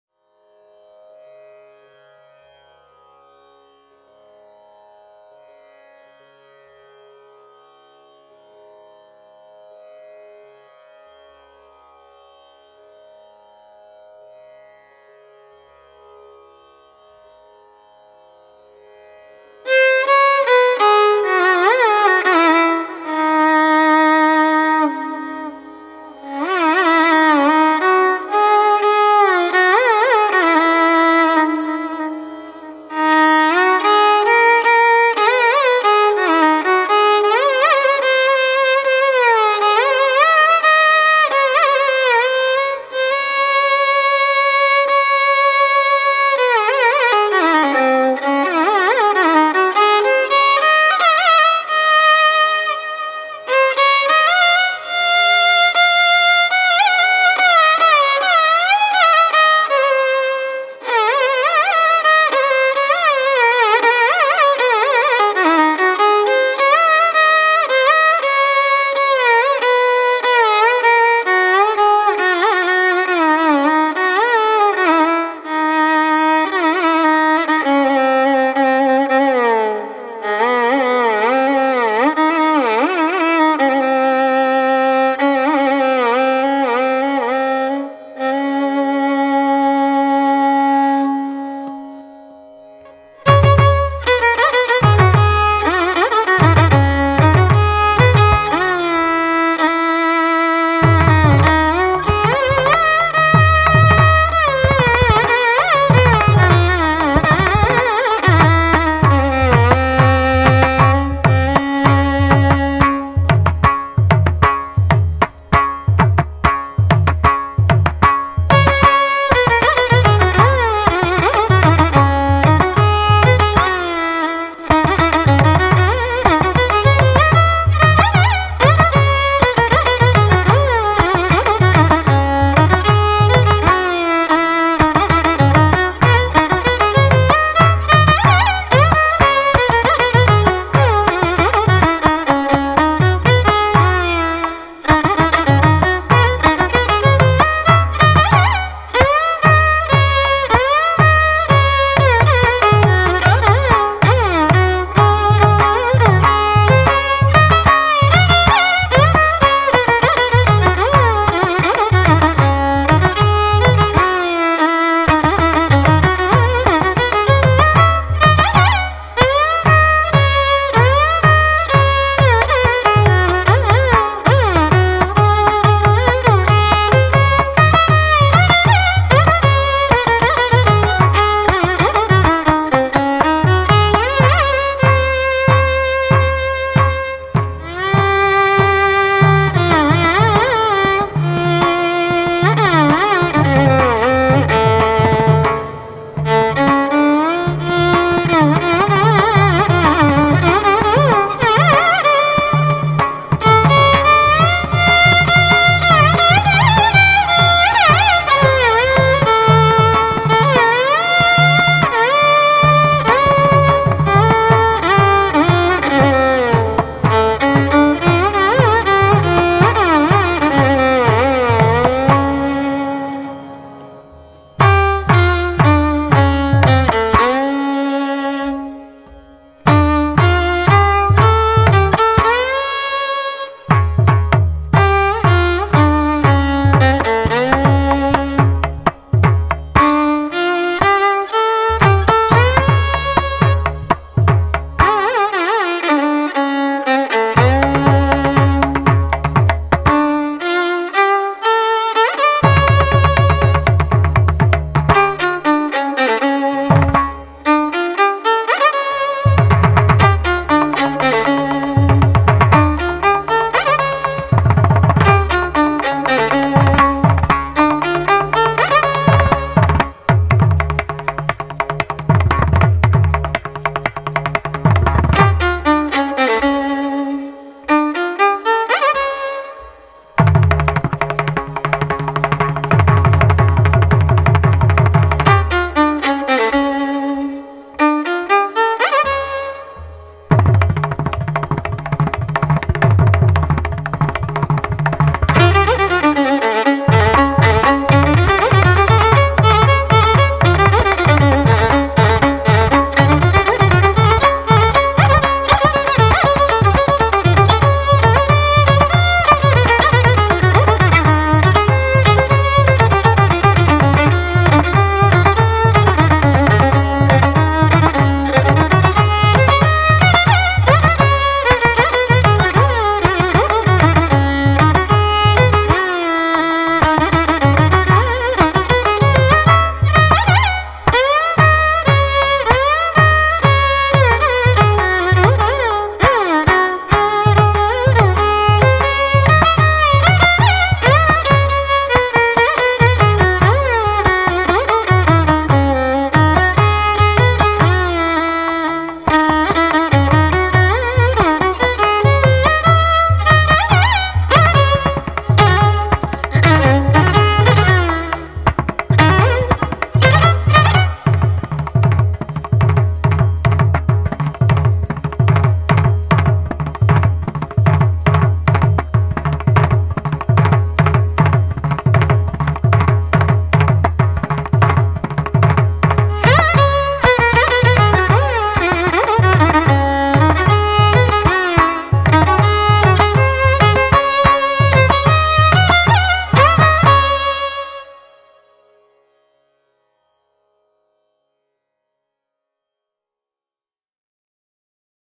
K. Saigiridhar and K.P. Nandini : An original composition.
An original composition in Dwi nadai - Dwi raga.
K.Saigiridhar-Mridangam,
Violin- K.P.Nandini
Mixing and Mastering : Shri Sainath
Video and Editing - Muthu Krishnan and Balaji Manivannan
Sai Rocks Studio